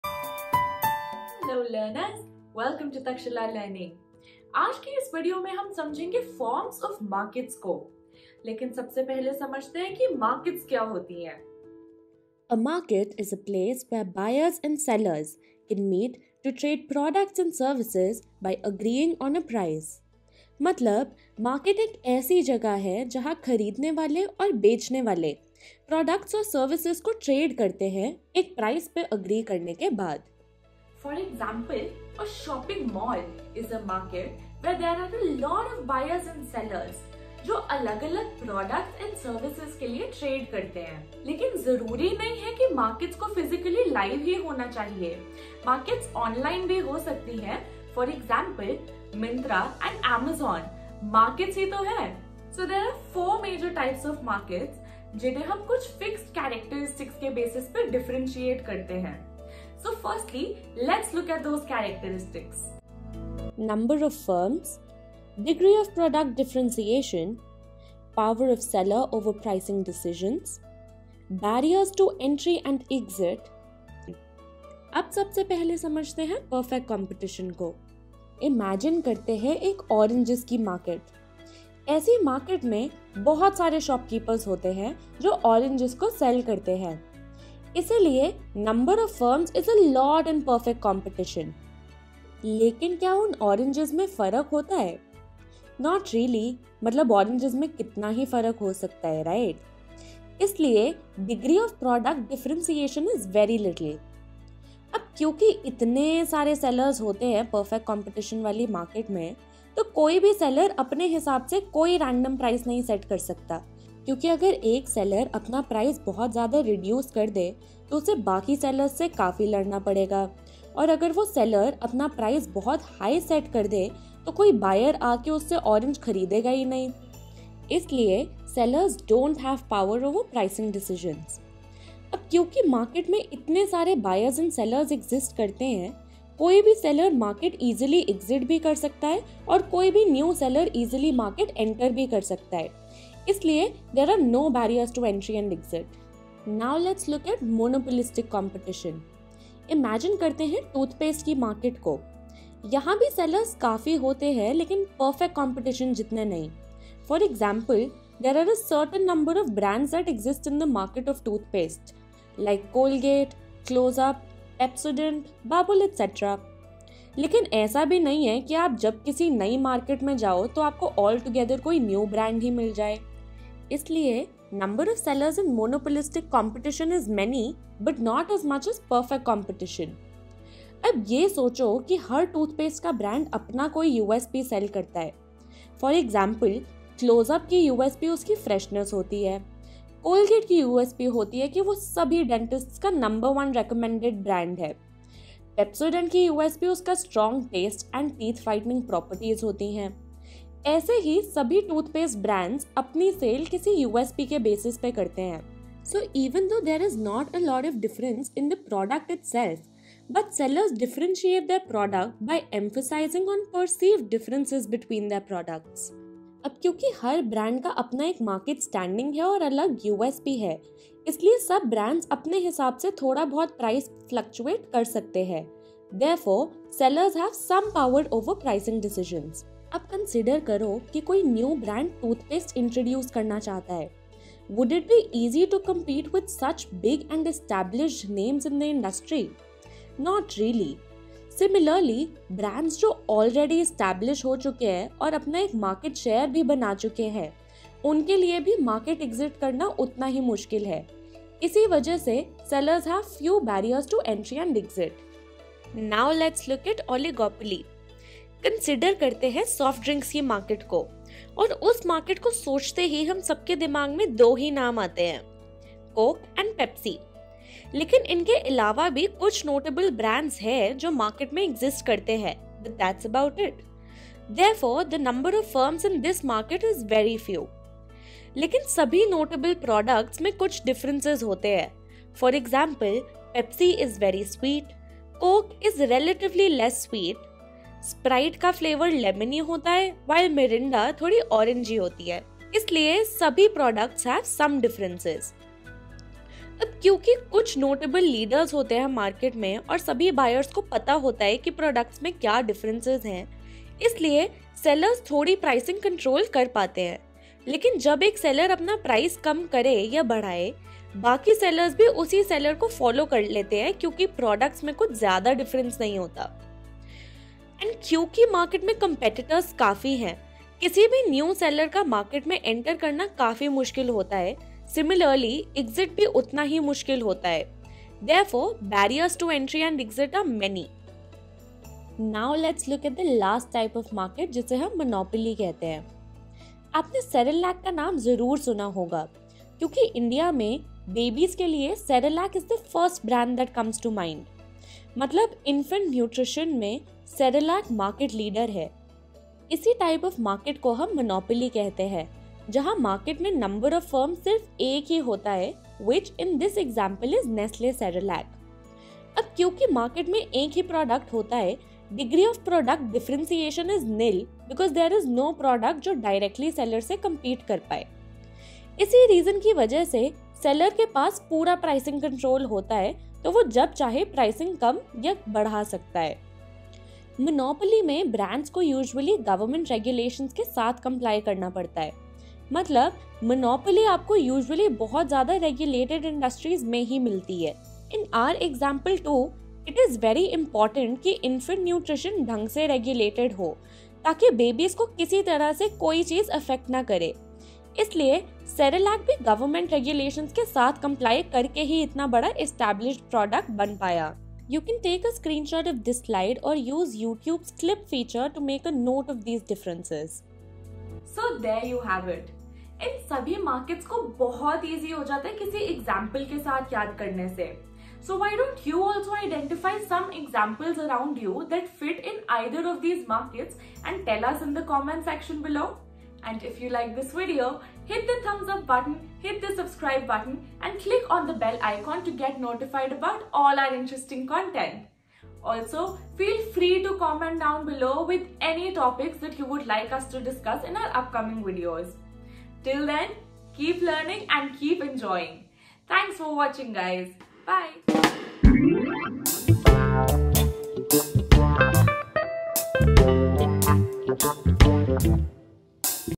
Forms of market class 11 economics | Micro Economics
In this video, we discuss the forms of markets based on the Class 11 Economics Chapter 9. We discuss the meaning of market and the 4 major types of markets that hold heavy mark weightage in the Class 11 Economics examinations. The 4 major types of markets are Monopolistic competition, Monopoly, Oligopoly and Perfect Competition. We differentiate these markets based on certain characteristics and understand them using examples.